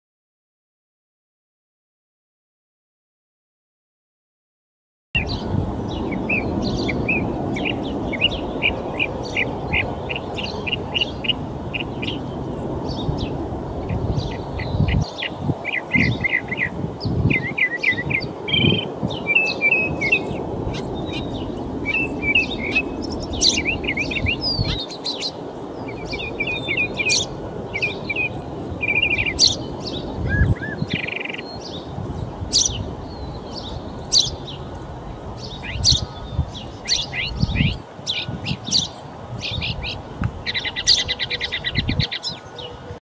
Mockingbird mimicking other bird songs. Mock2FF
Mockingbird mimicking other bird songs. Taken at a park in Folsom, California, March 21, 2044